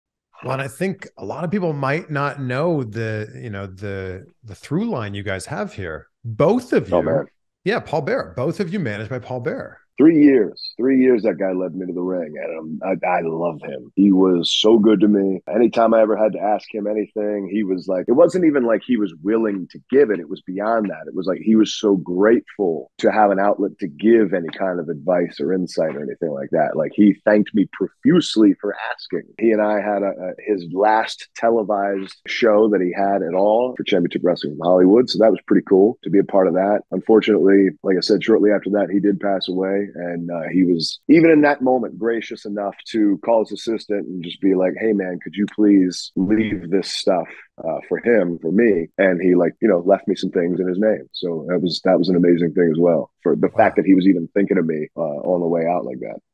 Paul Bearer Managed LA Knight
WWE Superstar LA Knight talks to Chris Van Vliet about being managed by WWE Hall Of Famer Paul Bearer